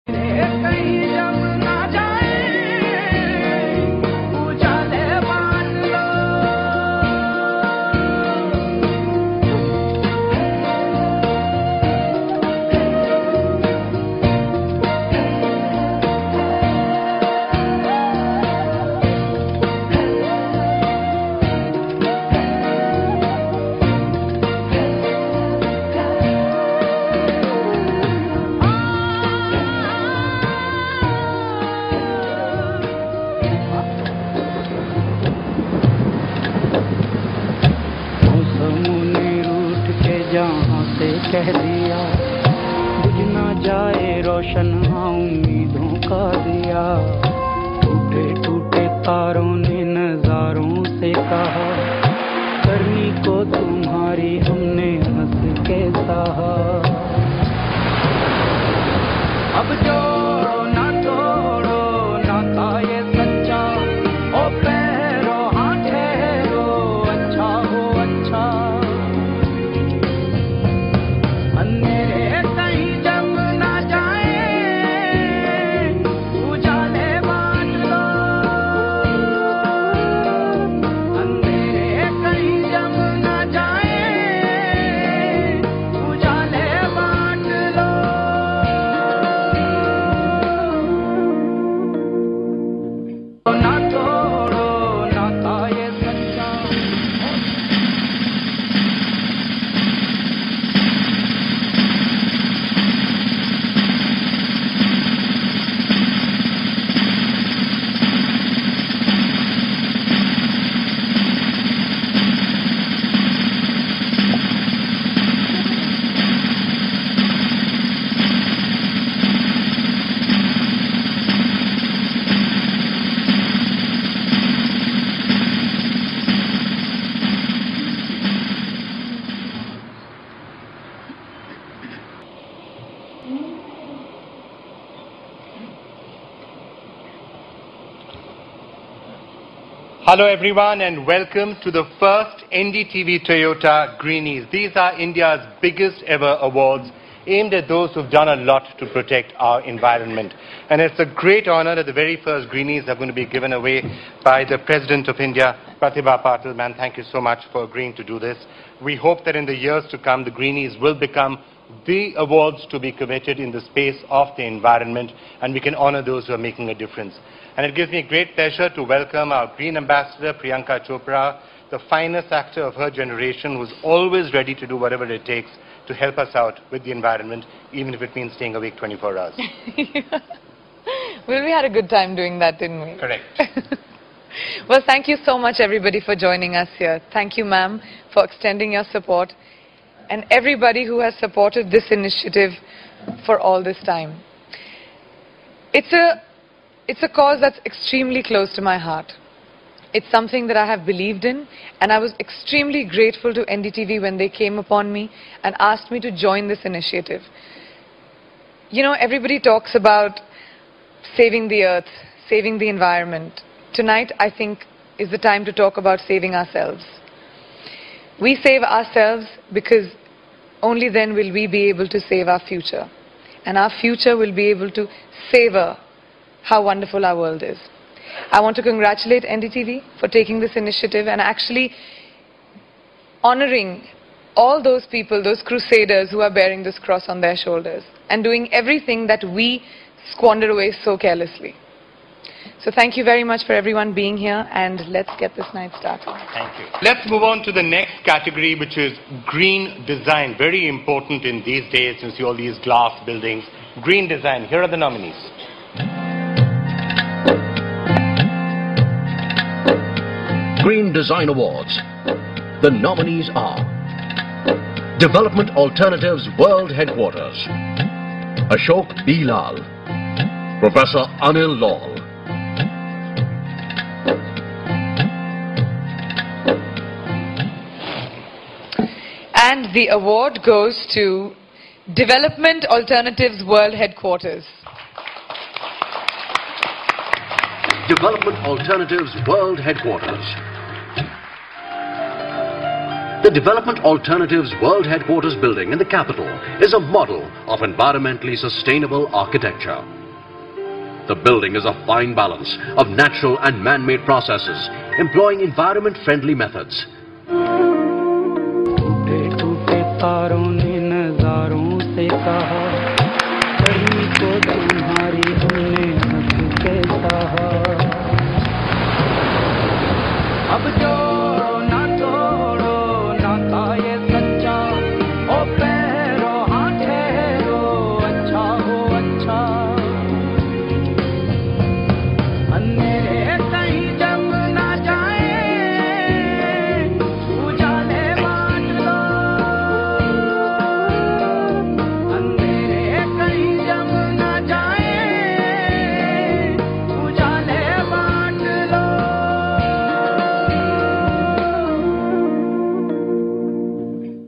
NDTV-Toyota Green Award | Year 2010 | Development Alternatives
Development Alternatives World Headquarters wins the NDTV-Toyota Green Eco Award for the Year 2010 in the category of Green Design.